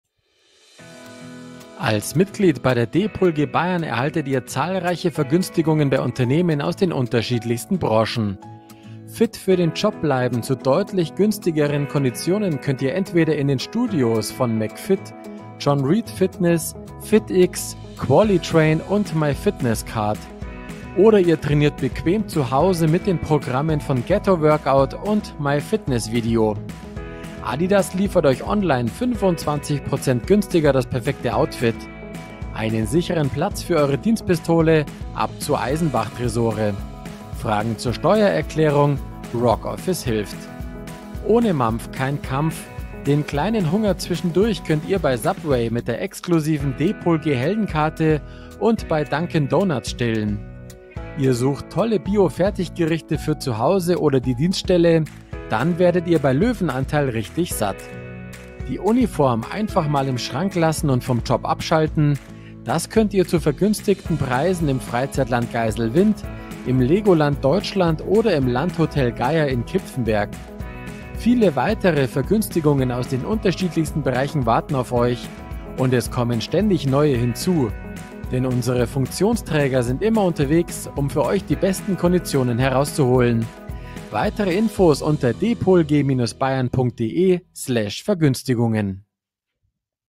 Sparen mit der DPolG Bayern
Als Mitglied bei der DPolG Bayern habt ihr die Möglichkeit, bei vielen Unternehmen aus den unterschiedlichsten Bereichen Geld zu sparen.

Exklusive Ermäßigung für DPolG-Mitglieder:
2,50 Euro Rabatt auf den Eintritt + 2,50 Euro Verzehrgutschein - Gilt nur bei Vorlage des DPolG-Ausweises und nur für den Inhaber selbst (keine Ermäßigung für weitere Personen)

Ghettoworkout:
Durch Deine DPolG-Mitgliedschaft kannst Du den Premium-Bereich der App kostenlos freischalten. Es stehen Dir dann weitere Übungen zur Verfügung.
Zudem bekommst Du einen 50%-Nachlass auf das Angebot der Live Video Workouts.
Den notwendigen Promotion-Code bekommst Du bei der Geschäftsstelle in München oder bei Deinem Bezirks-/Kreisvorsitzenden.
Freischaltung der Premium Workouts
Um das Bundle freizuschalten, gehe bitte wie folgt vor:
• Lade dir die Gettoworkout App aus dem App-/Play Store herunter
• Registriere dich in der App (mit Email oder Facebook, bei der Registrierung mit Facebook muss die Weitergabe der Email erlaubt werden (ist i.d.R. voreingestellt)
• Trage unten deine Registrierungsmail und deinen persönlichen Freischalt Code ein
• Absenden, App neu starten, fertig

Legoland Deutschland
Erleben Sie mit Ihren Liebsten einen unvergesslichen Sommertag voller Spaß und Abenteuer im LEGOLAND Deutschland Resort. Sie erhalten die 1-Tageskarten zu ermäßigten Preisen. Preis ist von der Vorbuchzeit abhängig.
Für die Buchung ist eine Kundennummer und ein Kennwort notwendig. Die Informationen gibt es beim jeweiligen Kreis- oder Bezirksvorsitzenden oder bei der Landesgeschäftsstelle.